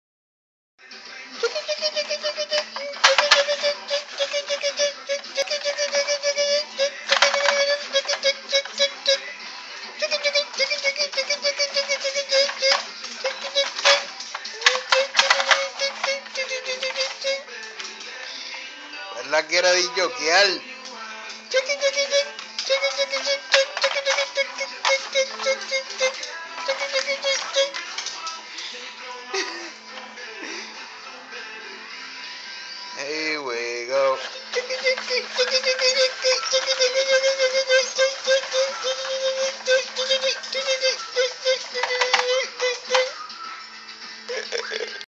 PERLA MINI SCHNAUZER DISK JOCKEY
PERLITA LOCA POR COMERSE SUS GALLETITAS Y YO MOLESTANDOLA, DITOOO!